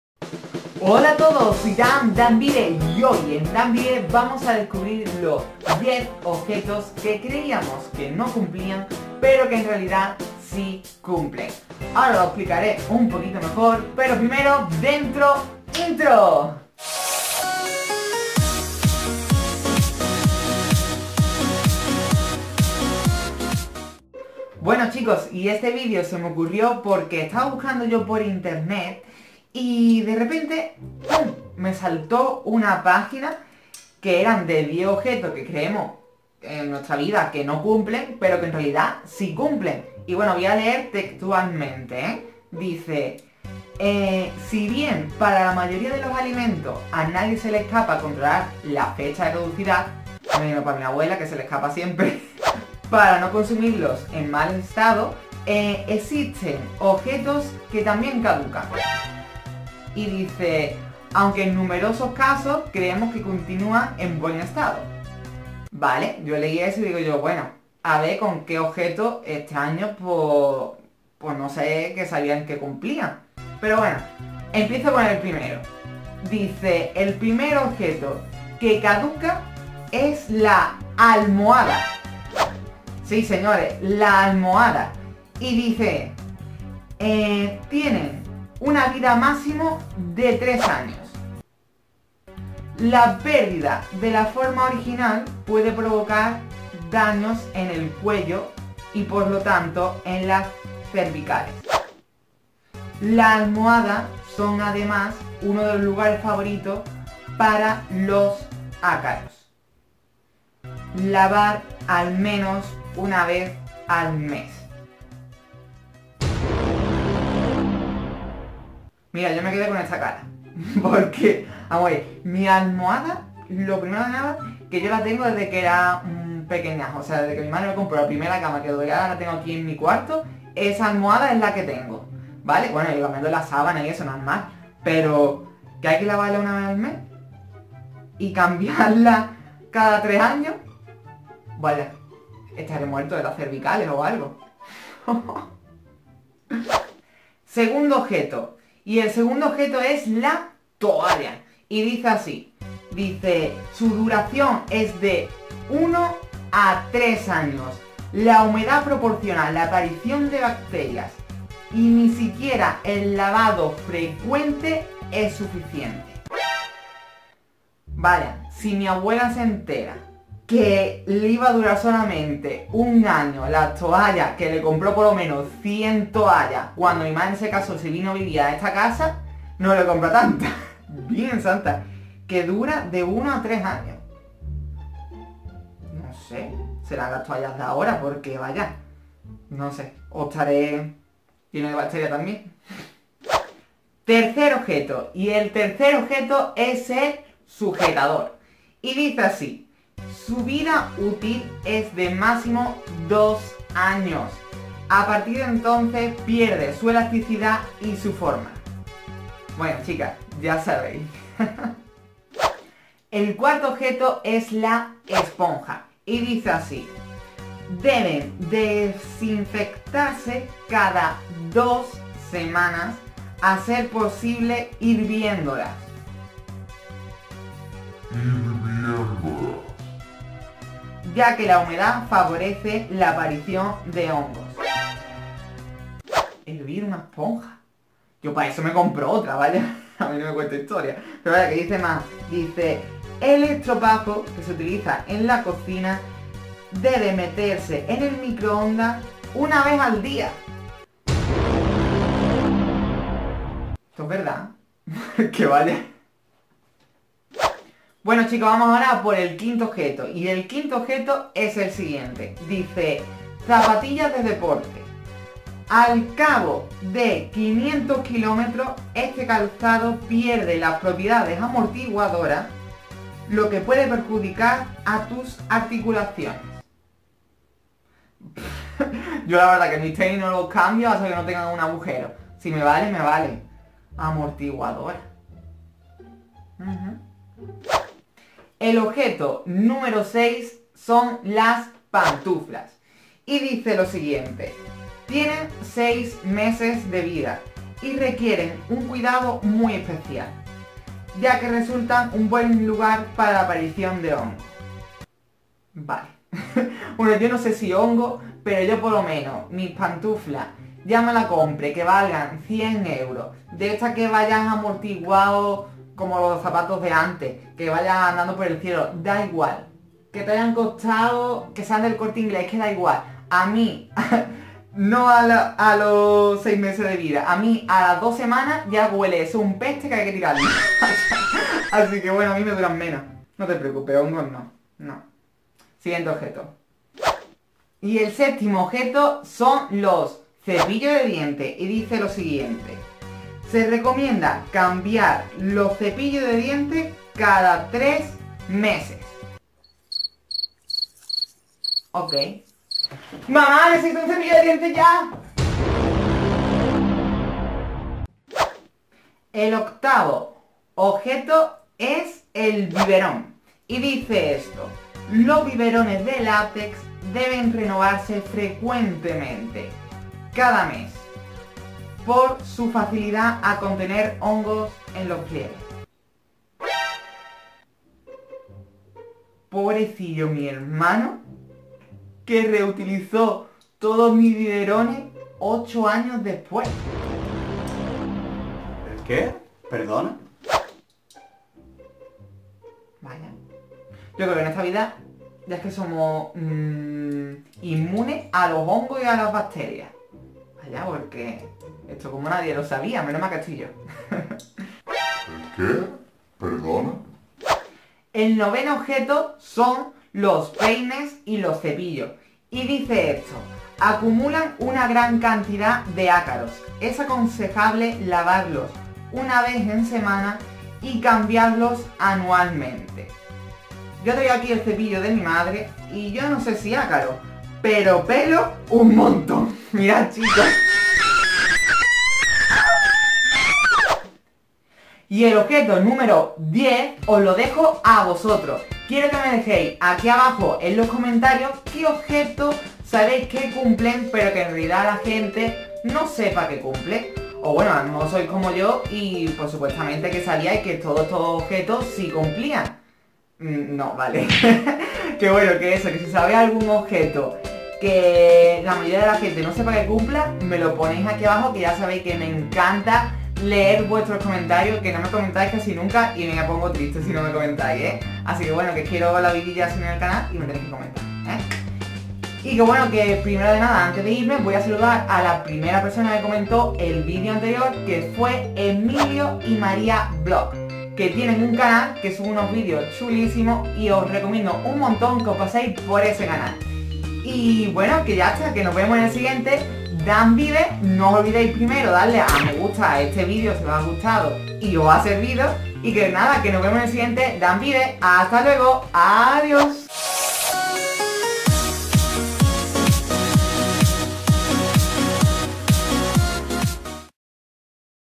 10 OBJETOS QUE PENSABAS QUE NO CUMPLÍAN | DAN VIVE VLOGS 😃
¡¡No te vas a creer la de objetos cotidianos que utilizamos normalmente que SI CUMPLEN!!
**Os recuerdo que la 1ª persona que comente este vídeo (y no haya sido ya saludada), lo saludaré en el siguiente vídeo😉
Málaga-Mijas Costa